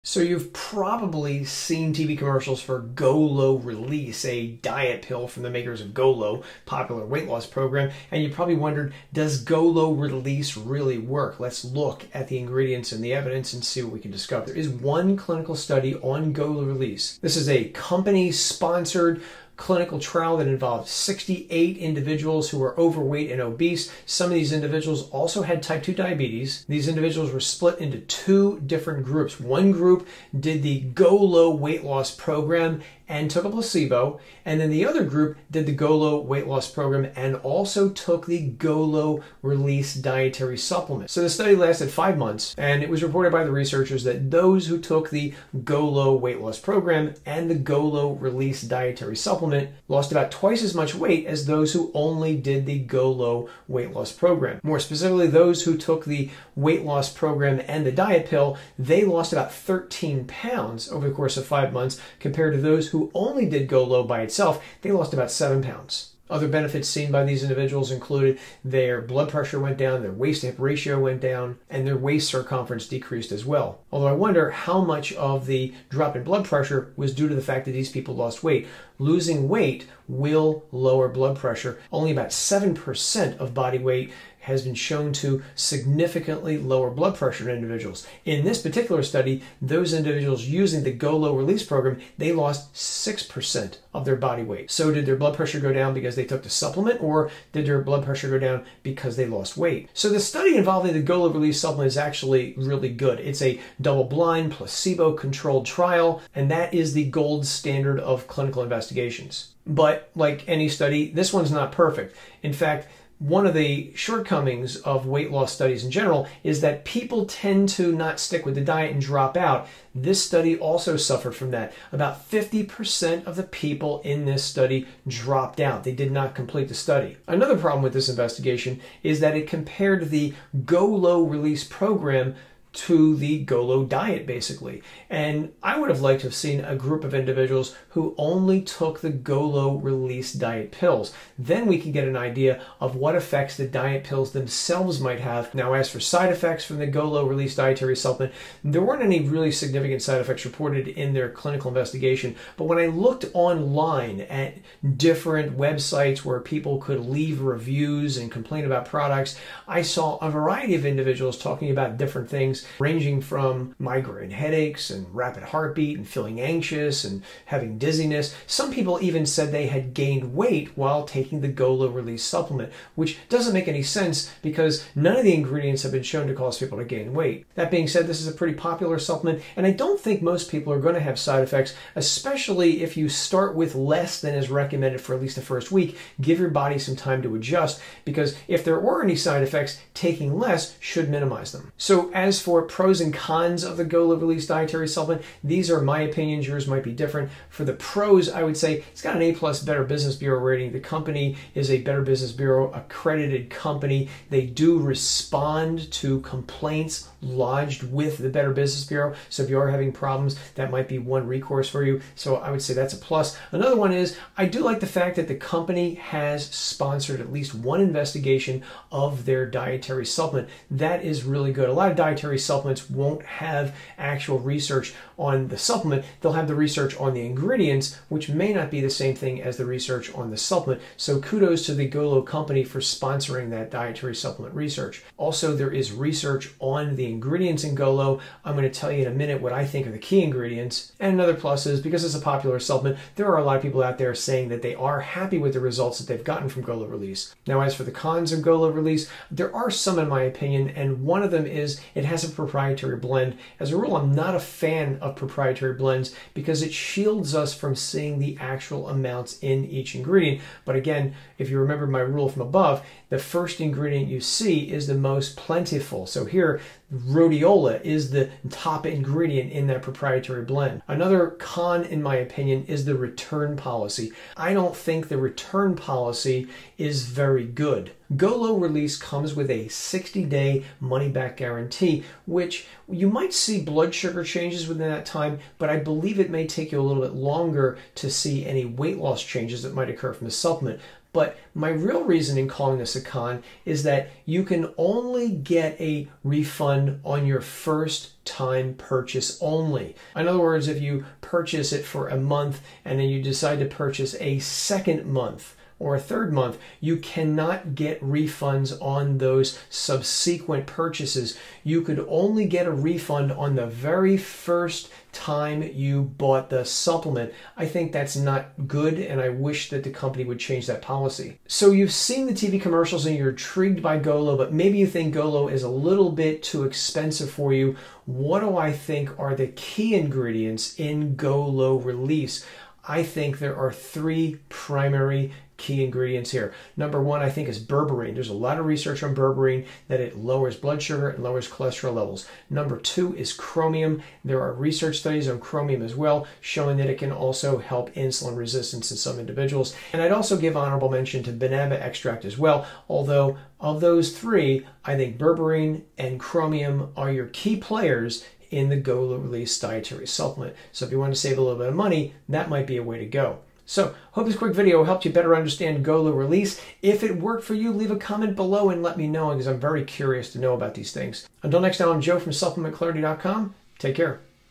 Does GOLO Release Help Insulin Resistance & Diabetes?
GOLO Release diet pills are popular for insulin resistance and weight loss; however, before you buy, there are things you need to know about, such as the proof, the key ingredients (which can save you money), and the GOLO refund policy just in case it doesn't work and you want your money back.

Video Chapters
00:00 Intro To GOLO Release Diet Pills
00:16 GOLO Diet Pill Clinical Study
01:52 Problems With The GOLO Study
02:33 GOLO Diet Pill Side Effects
03:16 Pros & Cons
04:36 GOLO Refund Policy
05:29 GOLO Diet Pills Key Ingredients (My Thoughts)

🔴🔴 🔴Things used to make this video:
1 Lot’s of work
2 This video editing software
3 This Microphone